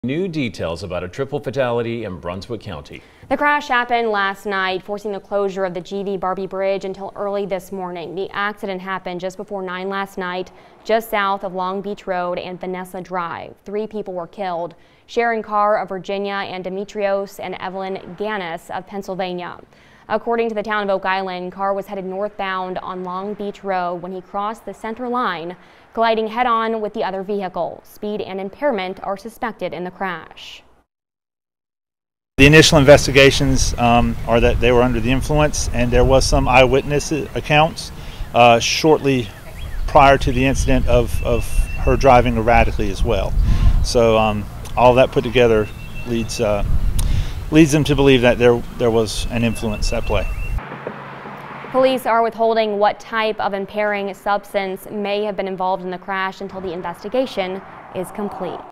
Victims identified in deadly Brunswick County crash
The Town of Oak Island has released the names of the people involved in a deadly crash Tuesday night.